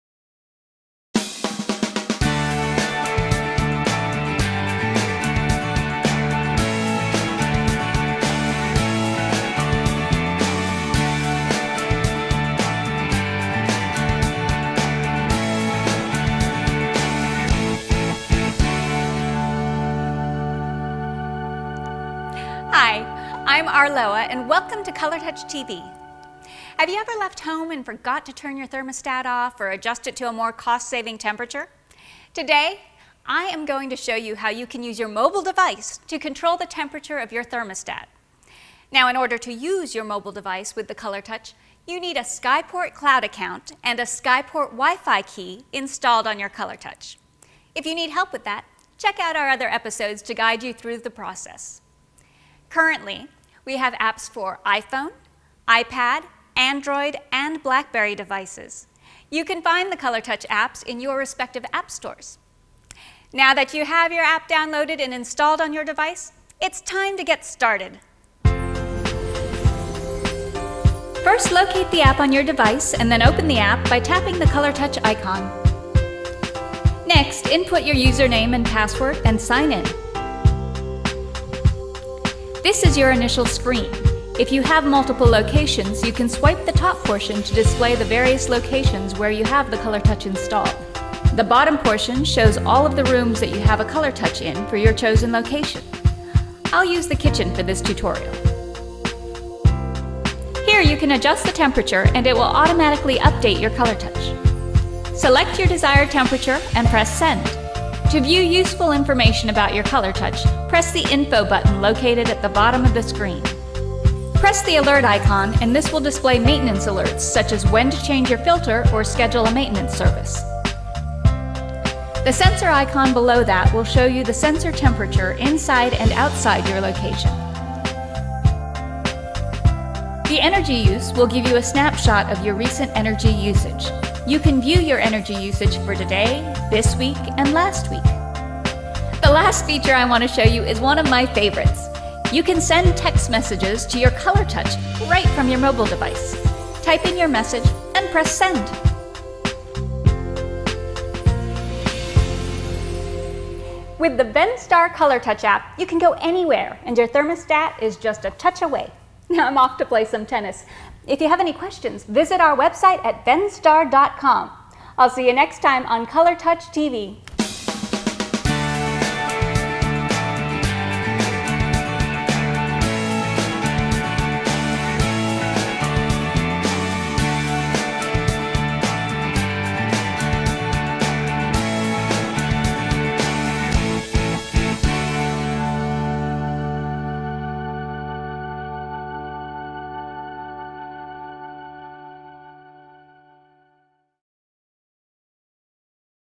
Venstar ColorTouch: Using the Mobile App
In this episode Arloa shows you how to use your mobile device to control your ColorTouch. She walks you through installing your app and setting it up on your mobile device.